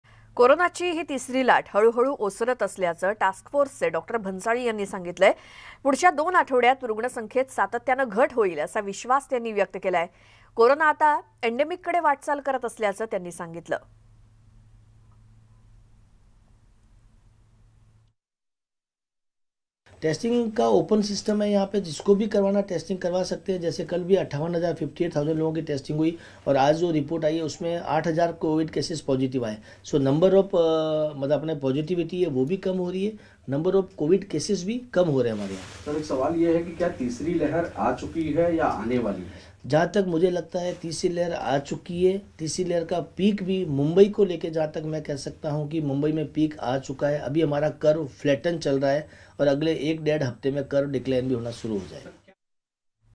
Corona Updates | कोरोनाची तिसरी लाट ओसरतेय, टास्क फोर्सचे सदस्य Dr.Gautam Bhansali यांची माहिती
कोरोनाचा संसर्ग आणि रूग्णांची संख्या येत्या दोन आठवड्यात झपाट्यानं कमी होणार अशी दिलासादायक माहिती टास्क फोर्सचे सदस्य गौतम भन्साली  (Dr.Gautam Bhansali)यांनी दिली.